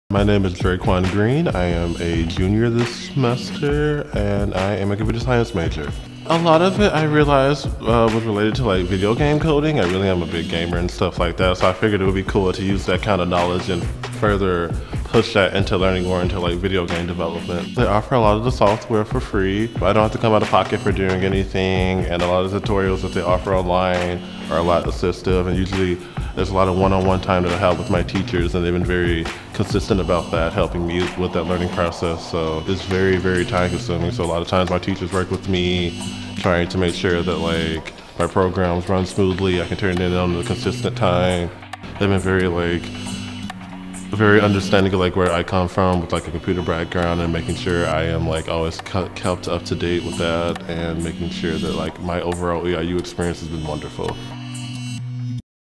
On-The-Spot Spotlight: Computer Science
For our "On-The-Spot Spotlight" video series, we catch up with randomly selected EIU students and talk to them about their studies as well as what they enjoy about their department and life in general at Eastern. In this installment, we interviewed DreQuan Green a Computer Science Major.